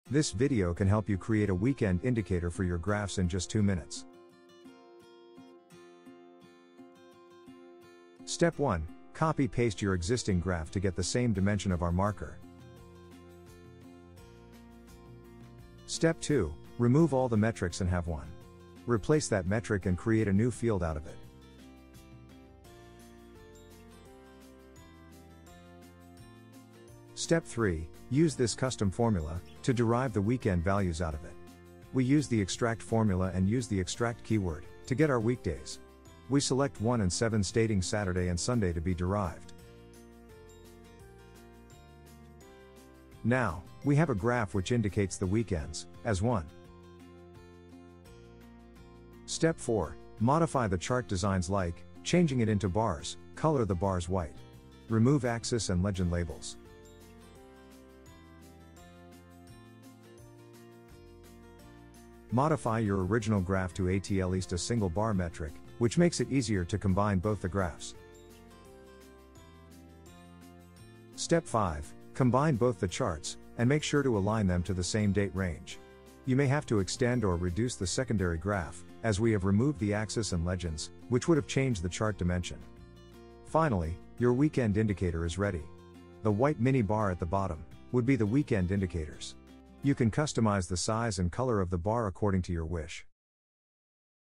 Weekend indicator for Time series Graphs in Looker Studio | Hack under 2 minute | Two Minute Reports
This is a detailed video, which guides you to create a weekend indicator for your graphs in Looker Studio under 2 mins. What are you waiting for...? Check out the video now!